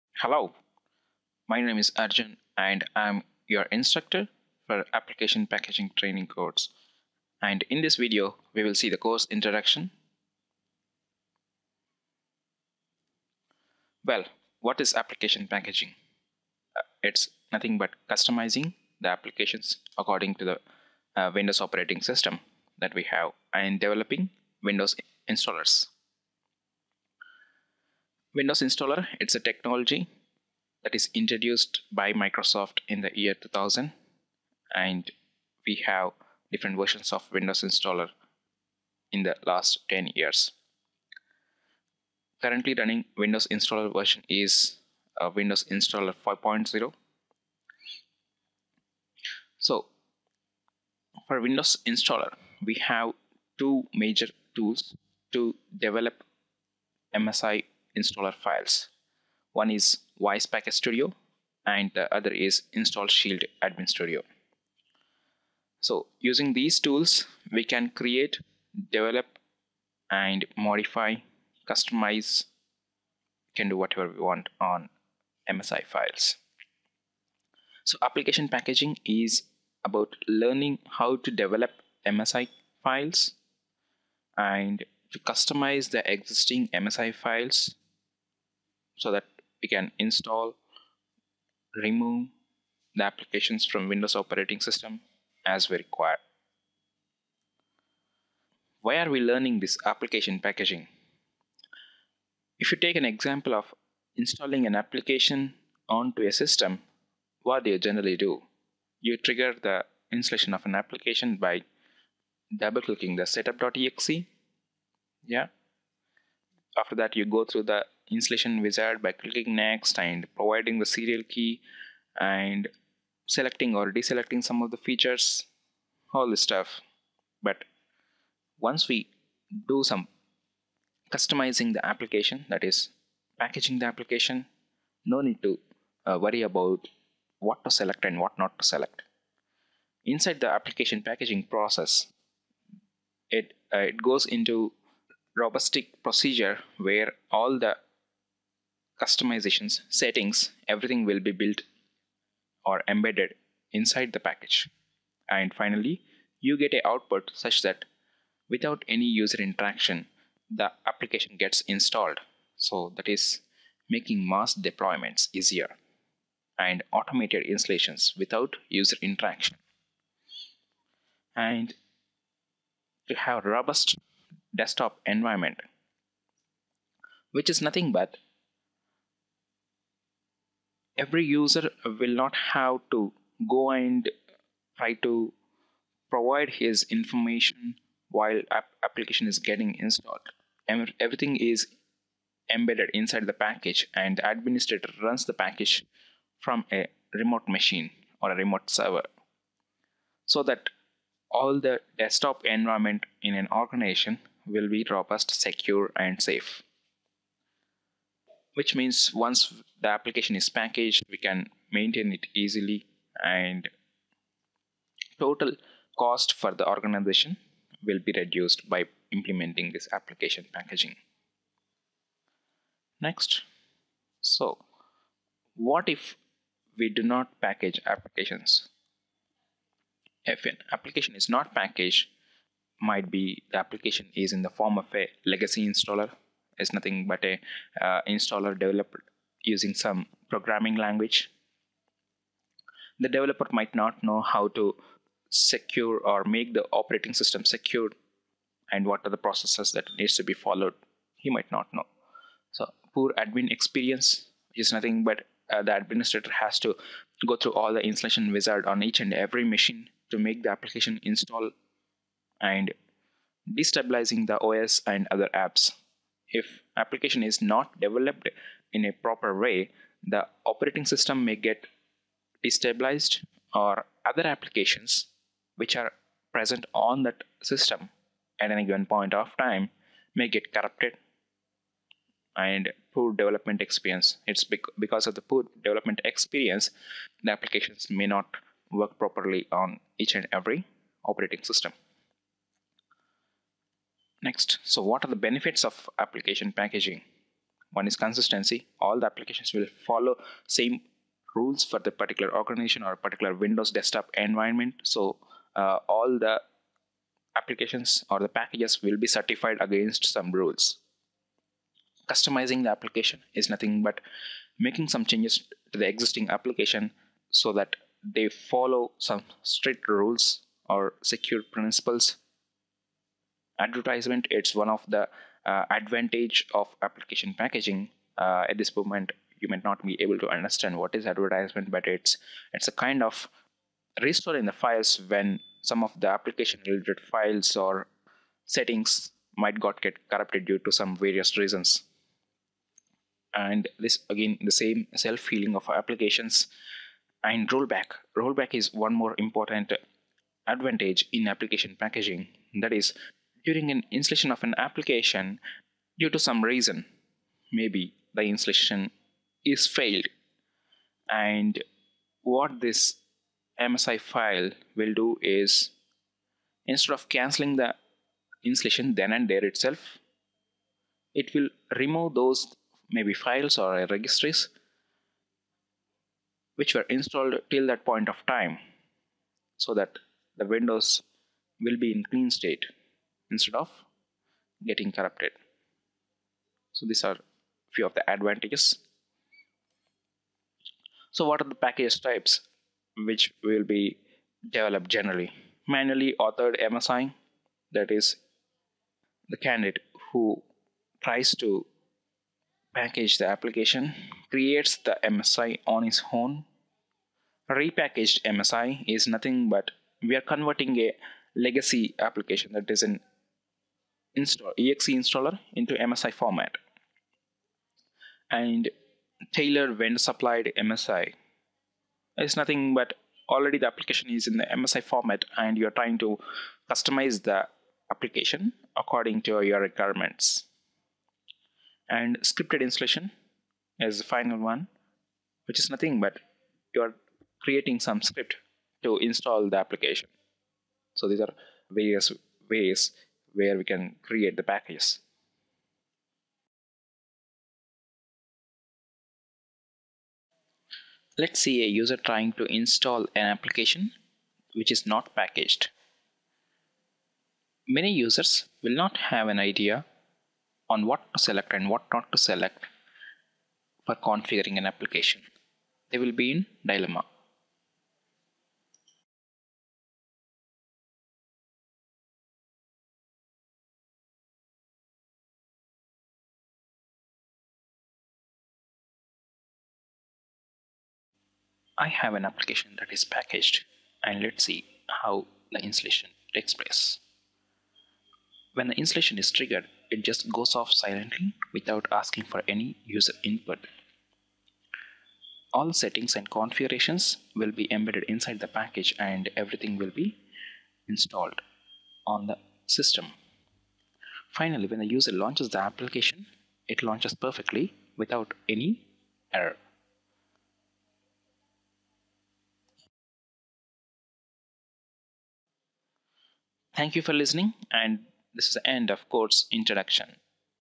Application Packaging Training Overview
This video gives introduction overview on Application Packaging